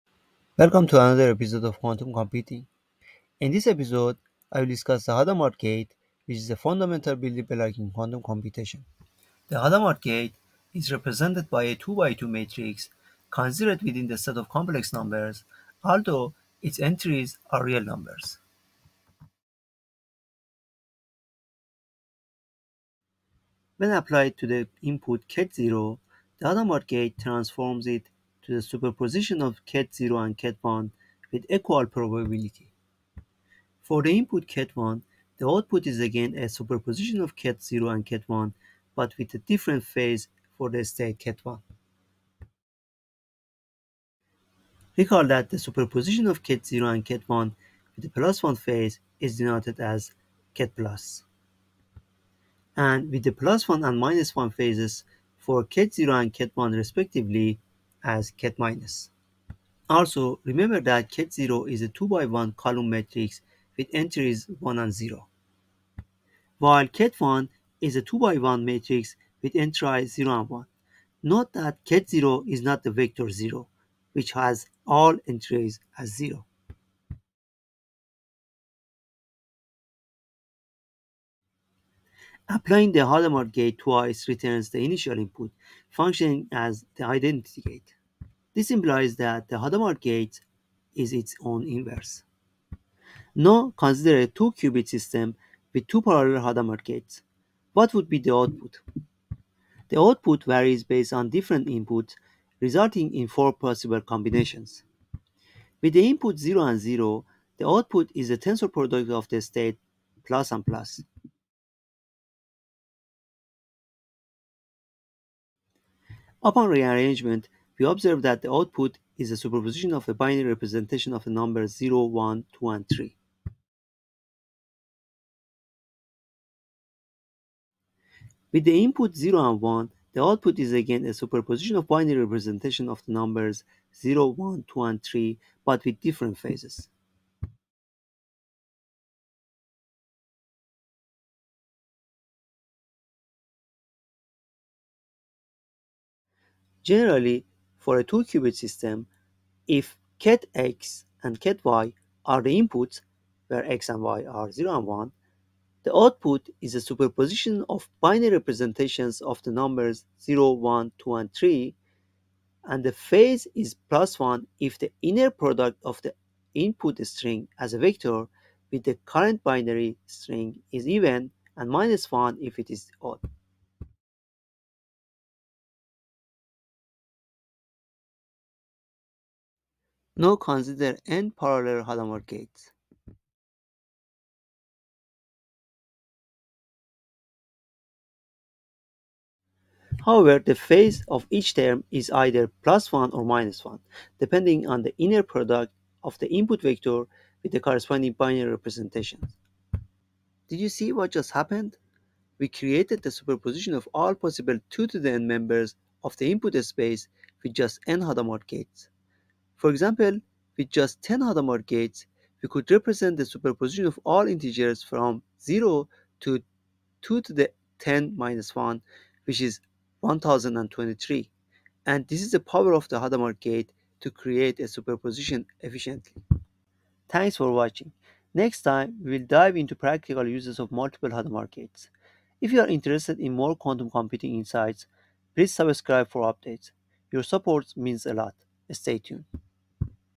Quantum Computing | Ep. 9: Hadamard Gate
Dive into the essentials of quantum computing with our concise guide to the Hadamard gate. Learn how it transforms quantum states and creates superpositions with just a few gates. Perfect for beginners and enthusiasts alike, this video demystifies a key quantum computing concept.